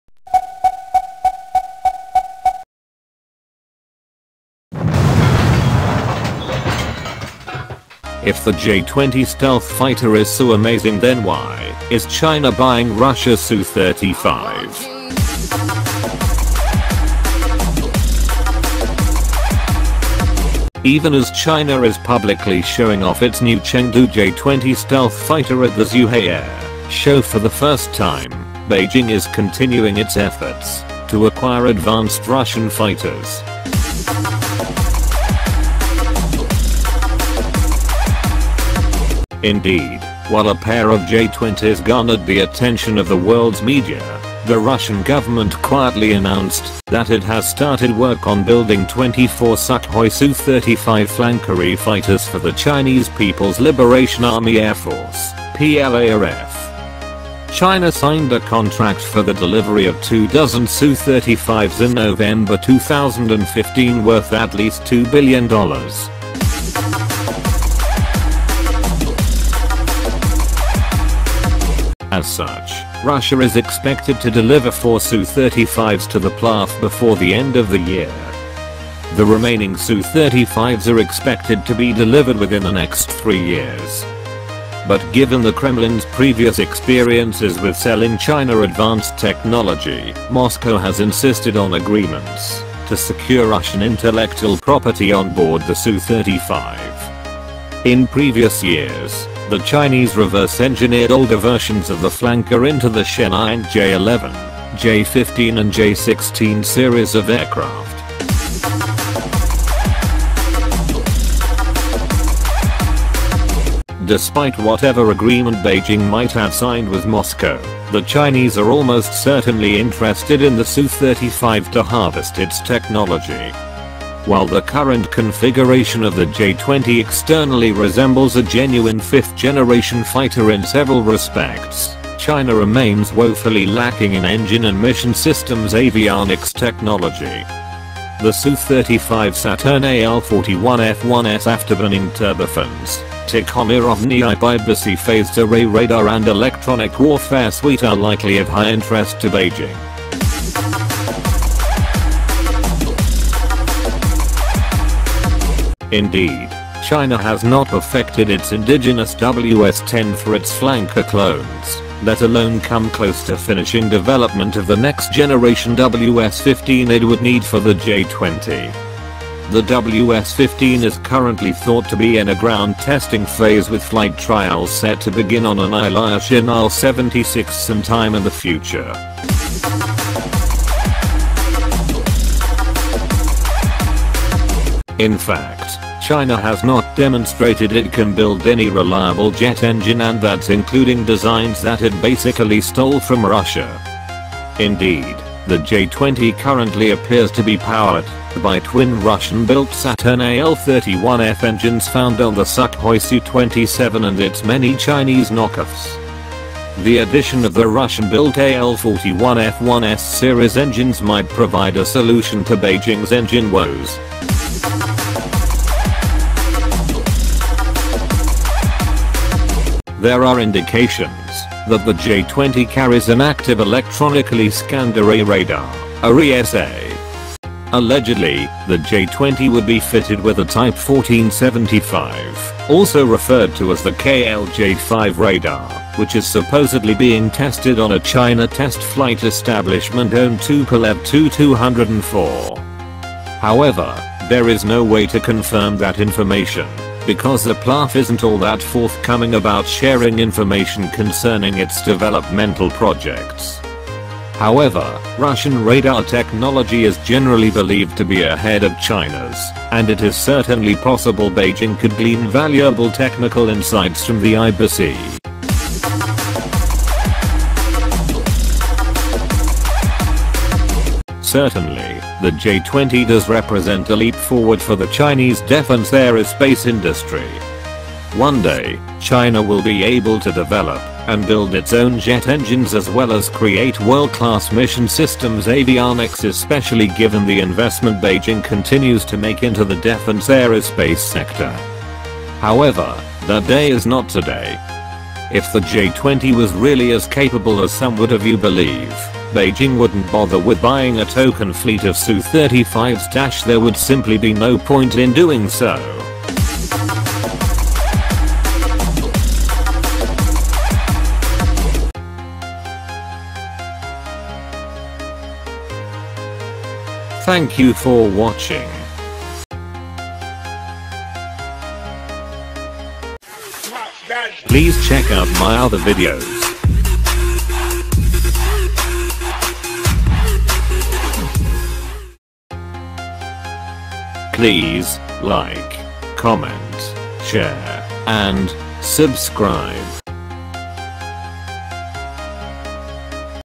If the J 20 Stealth Fighter Is So Amazing Then Why Is China Buying Russia's Su 35
This video show you that

Even as China is publicly showing off its new Chengdu J-20 stealth fighter at the Zhuhai air show for the first time, Beijing is continuing its efforts to acquire advanced Russian fighters.

Indeed, while a pair of J-20s garnered the attention of the world’s media, the Russian government quietly announced that it has started work on building 24 Sukhoi Su-35 Flanker-E fighters for the Chinese People’s Liberation Army Air Force (PLAAF). China signed a contract for the delivery of two-dozen Su-35s in November 2015 worth at least $2 billion.

As such, Russia is expected to deliver four Su-35s to the PLAAF before the end of the year. The remaining Su-35s are expected to be delivered within the next three years. But given the Kremlin’s previous experiences with selling China advanced technology, Moscow has insisted on agreements to secure Russian intellectual property onboard the Su-35. In previous years, the Chinese reverse engineered older versions of the Flanker into the Shenyang J-11, J-15 and J-16 series of aircraft.

Despite whatever agreement Beijing might have signed with Moscow, the Chinese are almost certainly interested in the Su-35 to harvest its technology. While the current configuration of the J-20 externally resembles a genuine fifth-generation fighter in several respects, China remains woefully lacking in engine and mission systems avionics technology. The Su-35’s Saturn AL-41F1S afterburning turbofans, Tikhomirov NIIP Irbis-E phased array radar and electronic warfare suite are likely of high interest to Beijing.

Indeed, China has not perfected its indigenous WS-10 for its Flanker clones, let alone come close to finishing development of the next-generation WS-15 it would need for the J-20. The WS-15 is currently thought to be in a ground-testing phase with flight trials set to begin on an Ilyushin Il-76 some time in the future.

In fact, China has not demonstrated it can build any reliable jet engine—and that’s including designs that it basically stole from Russia. Indeed, the J-20 currently appears to be powered by twin Russian-built Saturn AL-31F engines found on the Sukhoi Su-27 and its many Chinese knockoffs. The addition of the Russian-built AL-41F1S series engines might provide a solution to Beijing’s engine woes.

There are indications that the J-20 carries an active electronically scanned array radar (AESA). Allegedly, the J-20 would be fitted with a Type 1475 (also referred to as the KLJ-5 radar), which is supposedly being tested on a China Test Flight Establishment owned Tupolev Tu-204. However, there is no way to confirm that information because the PLAAF isn’t all that forthcoming about sharing information concerning its developmental projects. However, Russian radar technology is generally believed to be ahead of China’s and it is certainly possible Beijing could glean valuable technical insights from the Irbis-E.

Certainly, the J-20 does represent a leap forward for the Chinese defense-aerospace industry. One day, China will be able to develop and build its own jet engines as well as create world-class mission systems avionics—especially given the investment Beijing continues to make into the defense-aerospace sector. However, that day is not today. If the J-20 was really as capable as some would have you believe, Beijing wouldn’t bother with buying a token fleet of Su-35s—there would simply be no point in doing so.